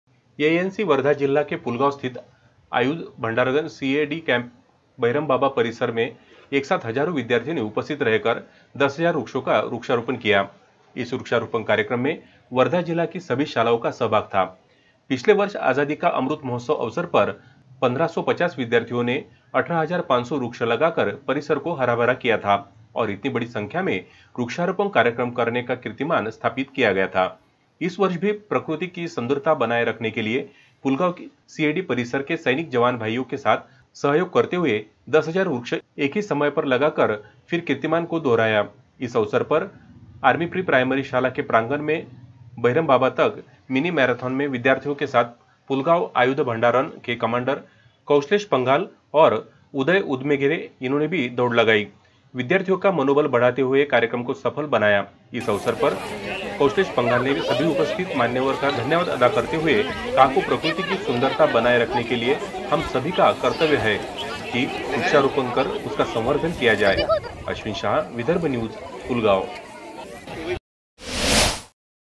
Pulgao -पुलगांव सी ए डी कैंप परिसर में आज 10000वृक्षों का वृक्षारोपण
विदर्भन्यूज365मराठी पुलगांव सी ए डी कैंप परिसर में आज 10000वृक्षों का वृक्षारोपण
किया विविध शाला के विद्यार्थियों ने

वृक्षारोपण के पश्चात मिनी मैराथन में दौड़े सैकड़ों विद्यार्थी

सी ए डी प्रशासन के कमांडर ब्रिगेडियर कोशलेश पंघाल और शहीद स्मारक समिति के उदय मेघ की विशेष उपस्थिति